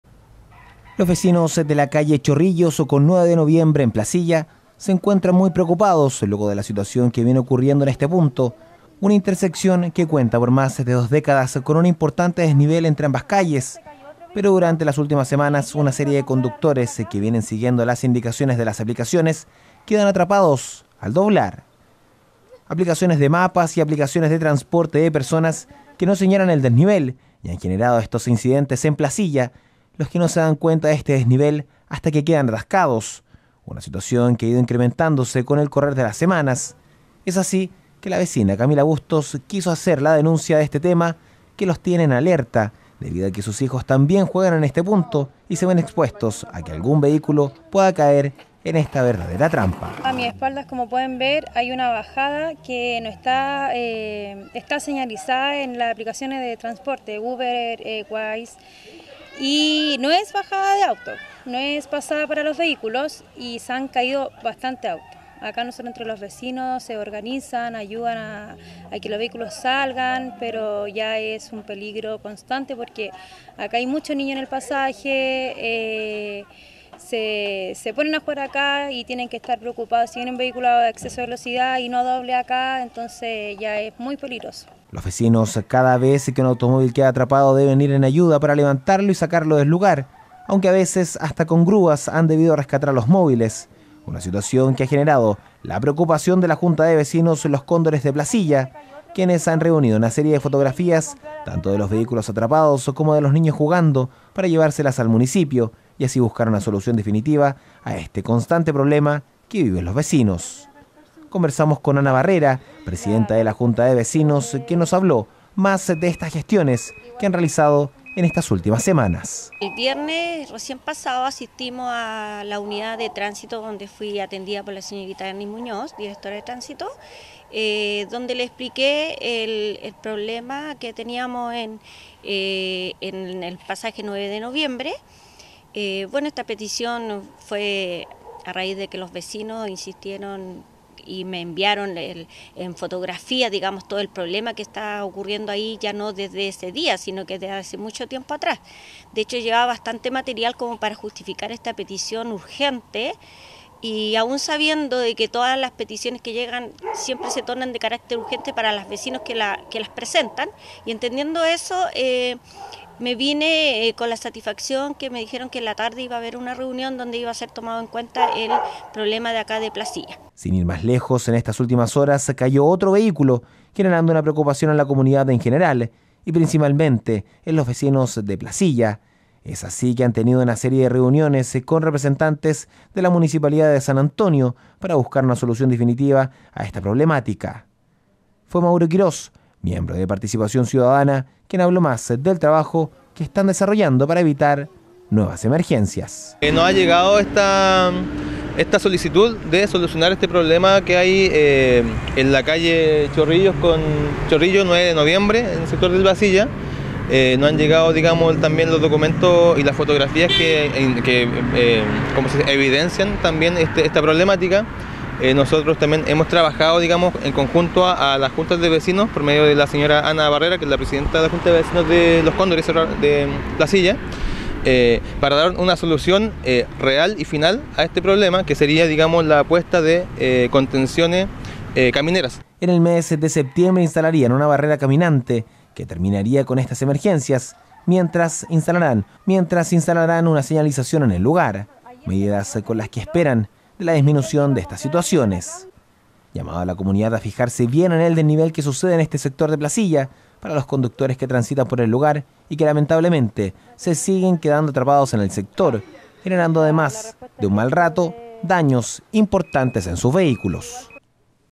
23 MAY 2023 Vecinos denuncian desnivel del terror para decenas de automovilistas
Una particular situación viene ocurriendo hace semanas en Placilla, debido al desnivel que hace más de 2 décadas presenta la calle Chorrillos y 9 de noviembre, pero las aplicaciones de mapas y transporte de aplicaciones no detectan el desnivel, lo que ha hecho caer a decenas de conductores en esta trampa que deja atrapados. Fueron los propios vecinos denunciaron y espera se pueda reparar pronto. Conversamos con la comunidad y los vecinos y esto es lo que nos comentaron.